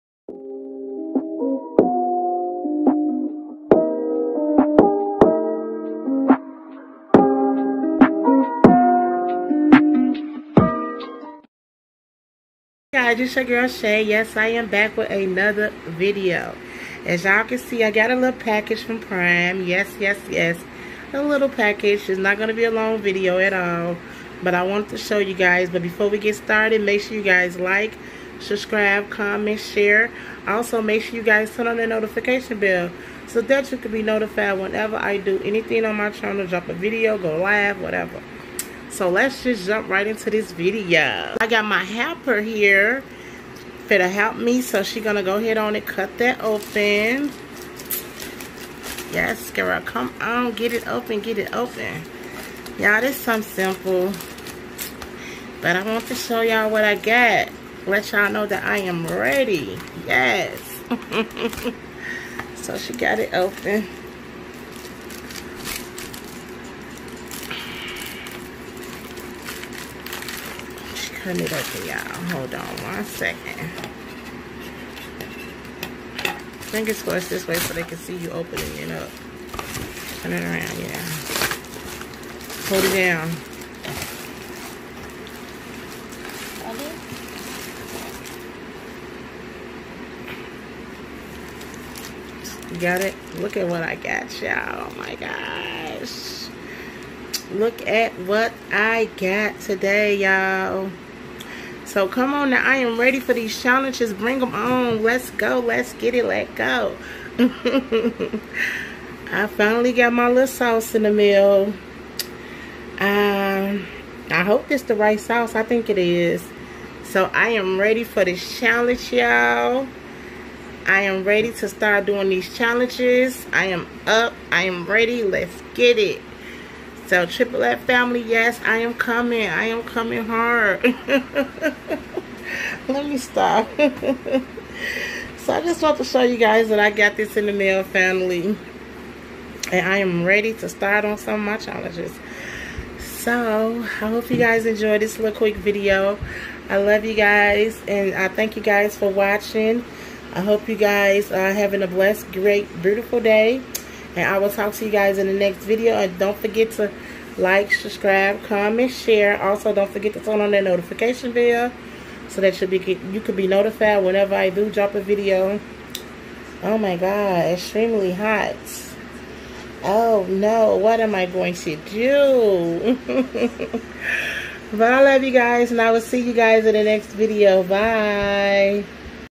Quick Amazon Prime Unboxing!!
Hey guys so i finally ordered  my 2× spicy sauce from @AmazonPrime and I'm sooo excifor these challenges.  The price was very reasonable so if you would like to get some here is the link